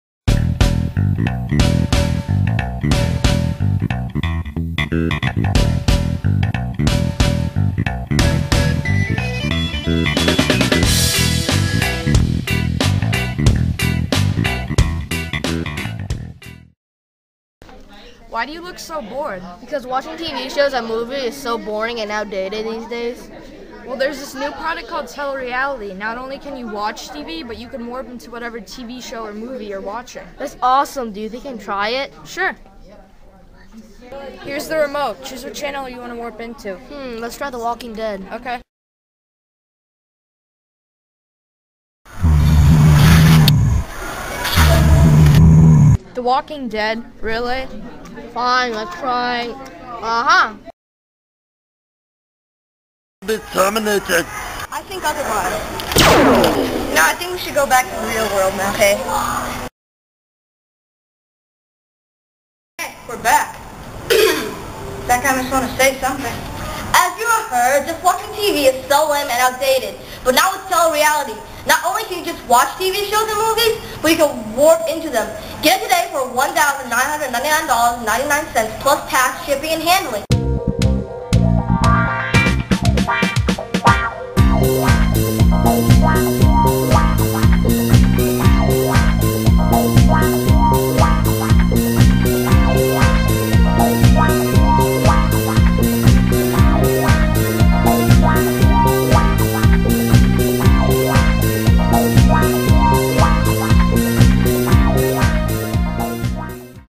TeleReality - Invention By Liudvikas S, Steven W, Lian W.
This awesome invention allows you to warp into whatever TV show or movie you are watching at the press of a button. Only $1999.99 plus tax, shipping and handling...